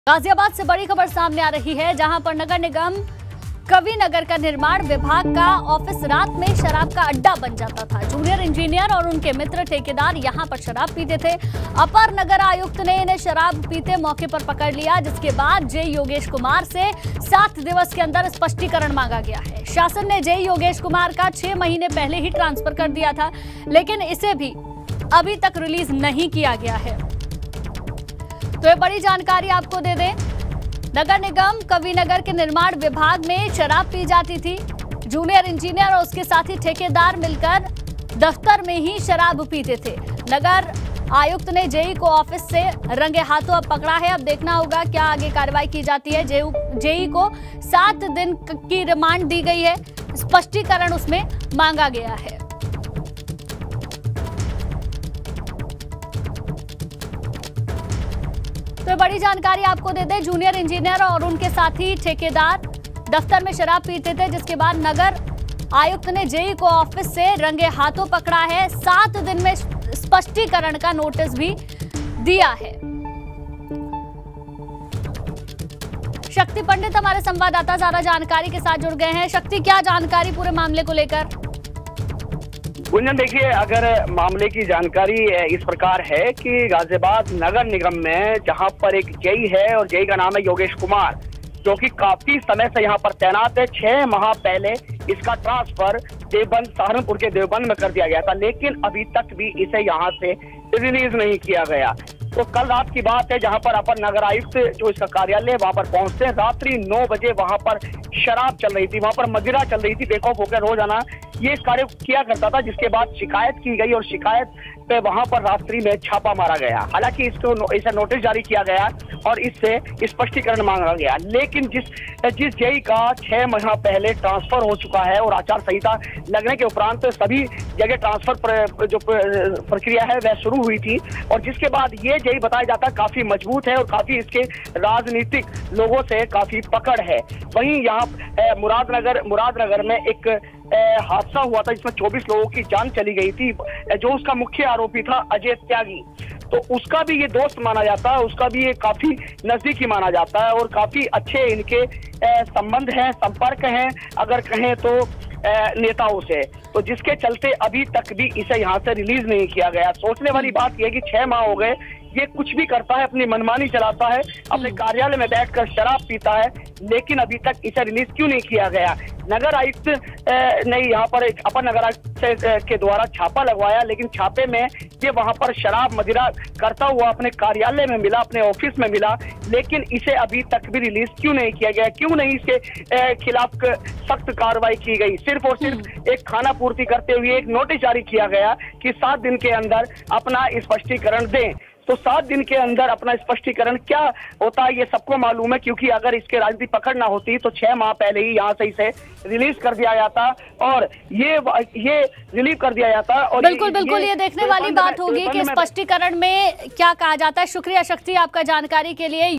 सरकारी ऑफिस रात में बन जाता था शराब का अड्डा, अब जेई ने मांग जवाब
गाजियाबाद से बड़ी खबर सामने आ रही है जहां पर नगर निगम कविनगर का निर्माण विभाग का ऑफिस रात में शराब का अड्डा बन जाता था जूनियर इंजीनियर और उनके मित्र ठेकेदार यहां पर शराब पीते थे अपर नगर आयुक्त ने इन्हें शराब पीते मौके पर पकड़ लिया...